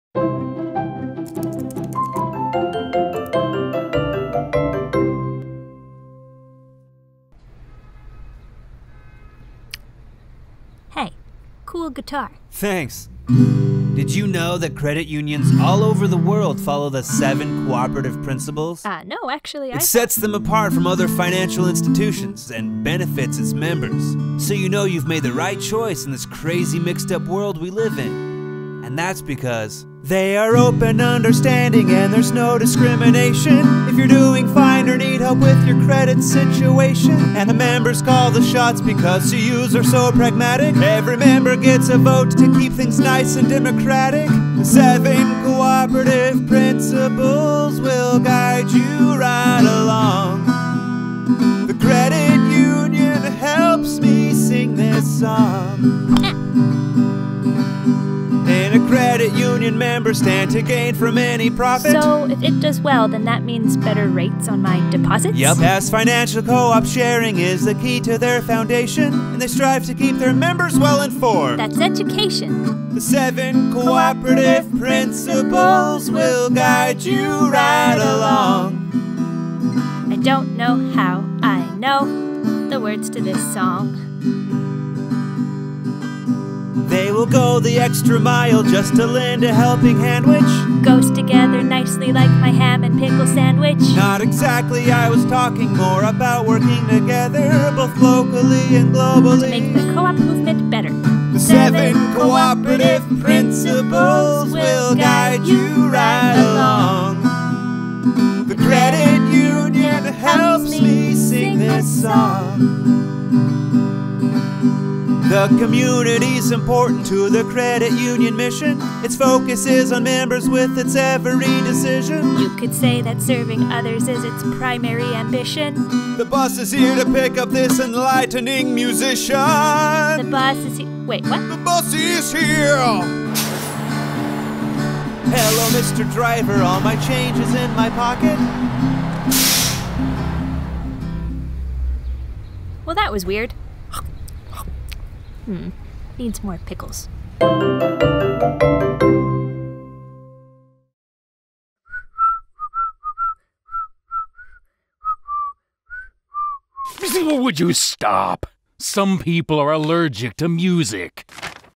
A Cooperative Financial Institution
Let's go go deep into credit union history and the guiding principles that make Healthcare Systems FCU unique.